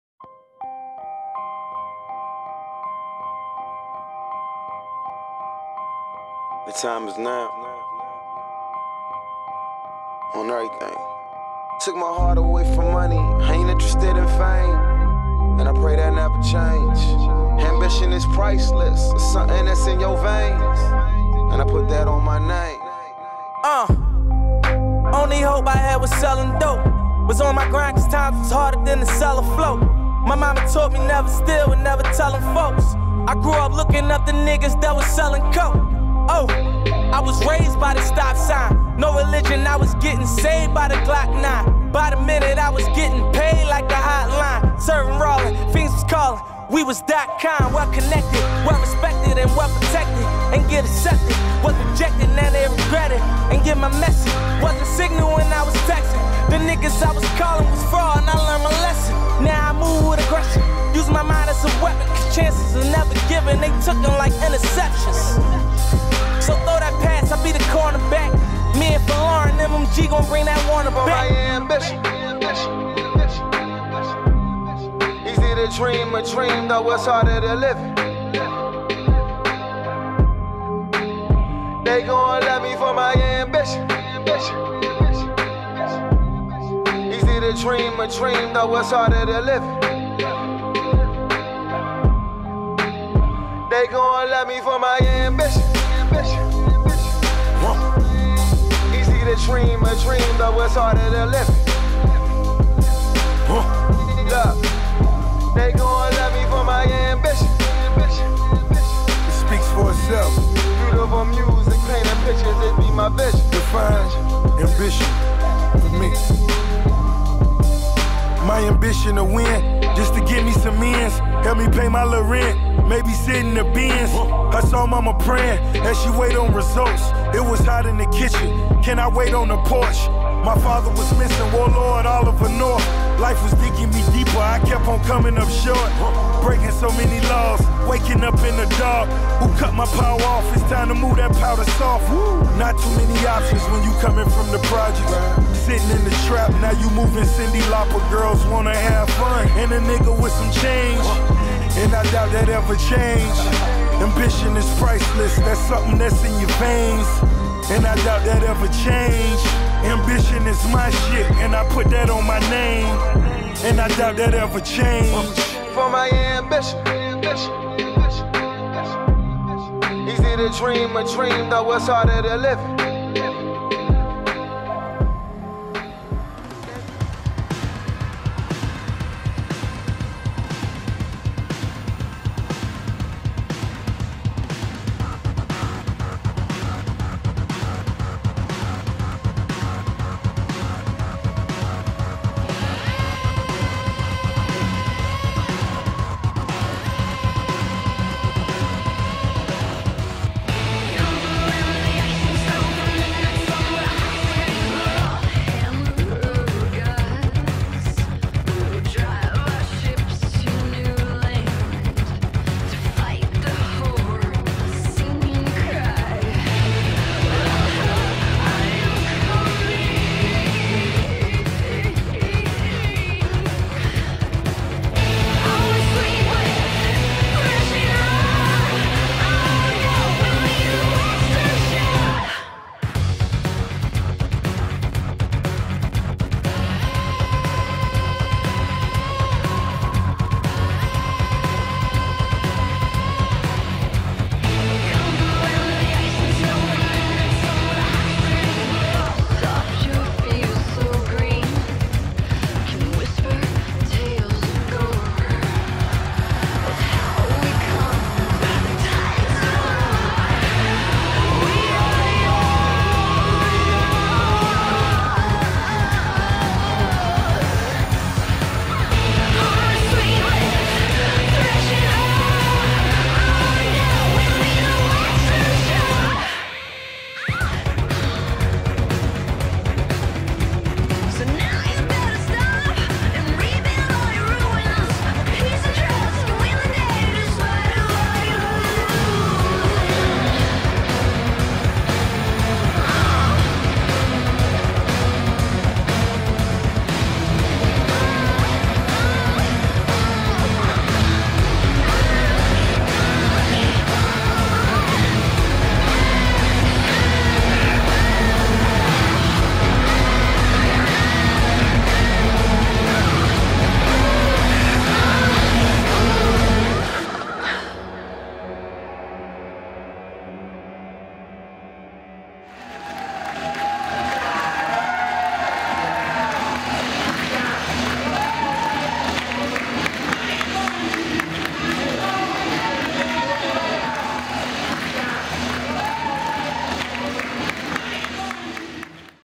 Westside Kickboxing - Ambition (2012 tournament reel)
June 2 at Proving Ground muay thai tournament
Links to full fights at end of video
Videography by Alex Poutiainen